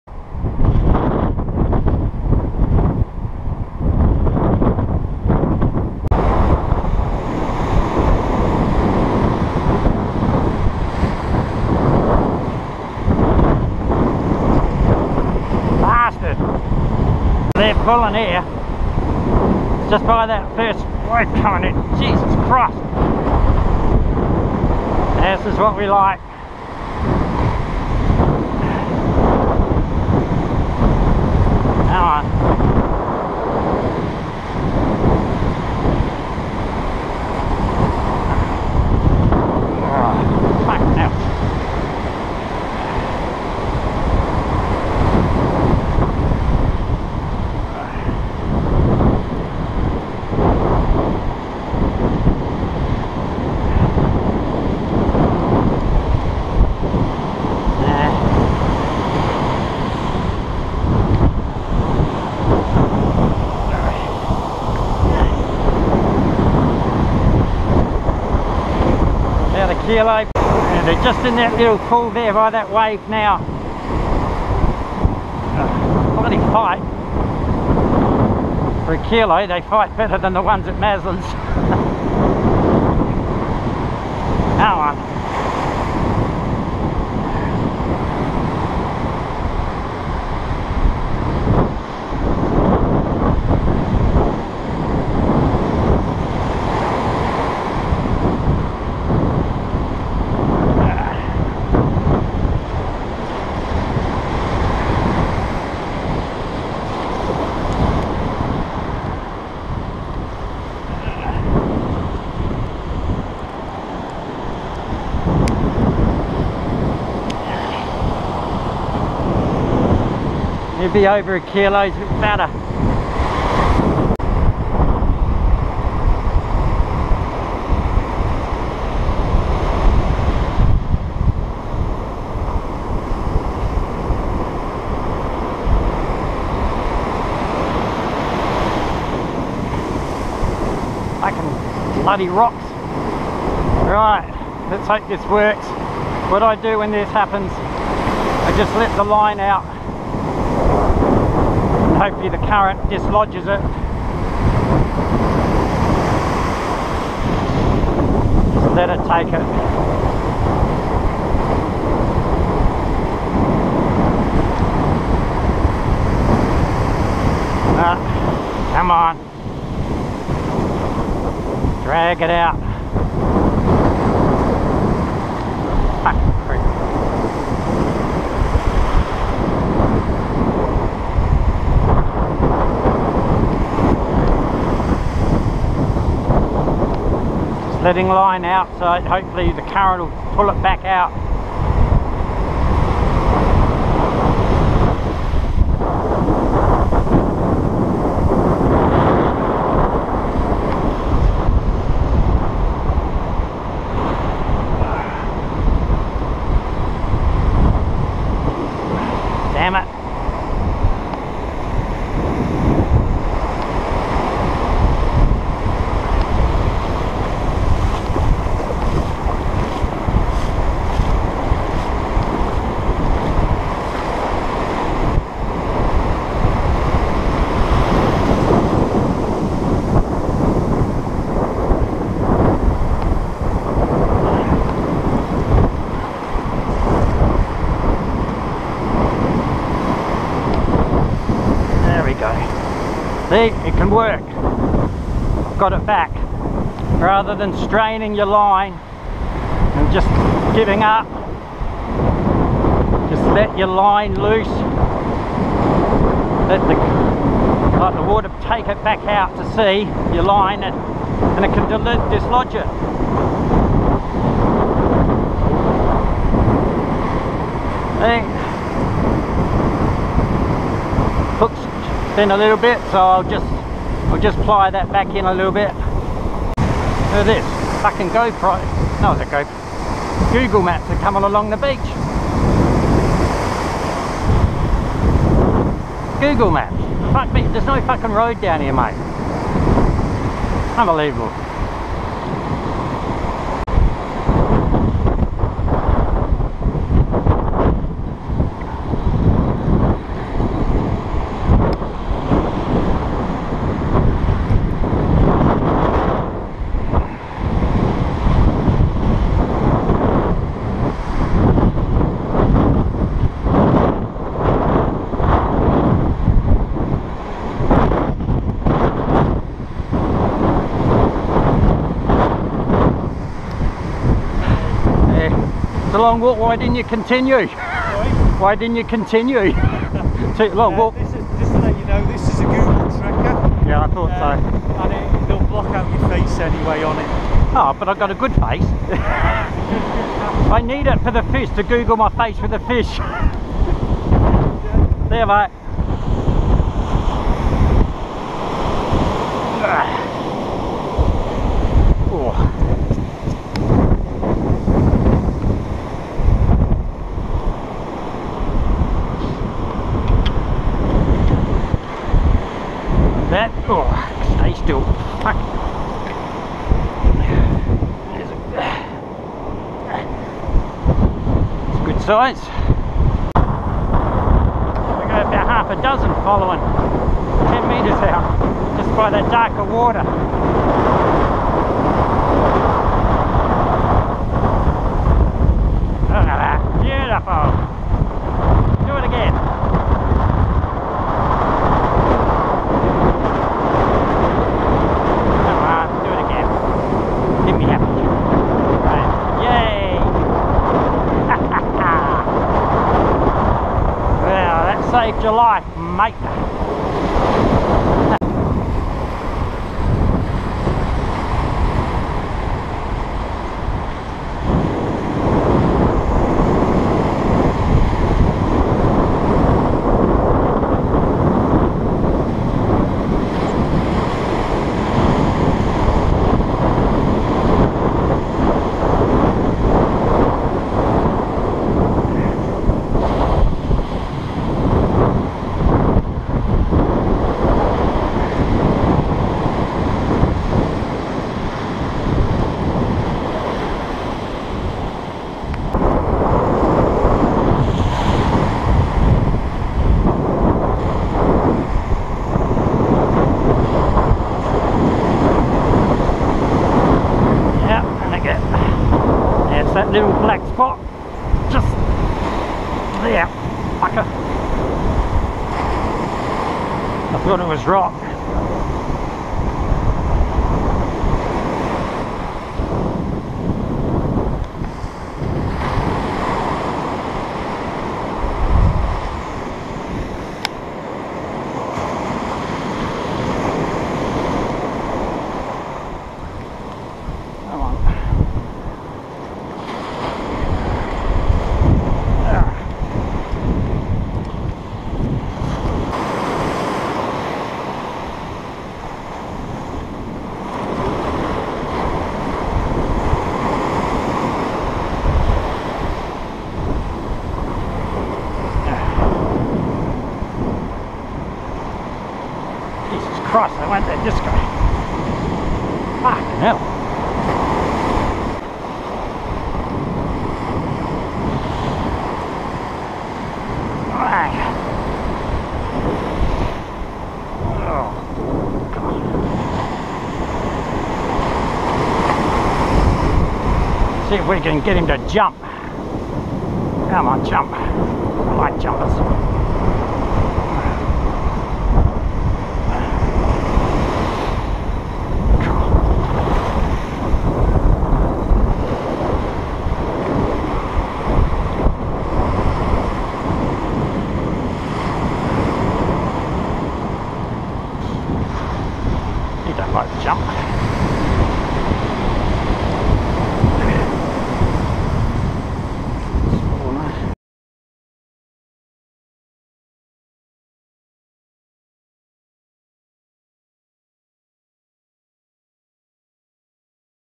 Parsons Beach (SA) 5.8.2019 (includes Google Map Tracker on beach & how to retrieve a snagged hook)
Windy conditions from the NW
A total of about 1kg Salmon all of which gave a good fight.
Saw a man come along the beach which I thought had a backpack on, but was the Google Map man walking the Heysen Trail.
Also got my lure snagged in a rock and will show you how I can retrieve it (have done a few times now and it usually works).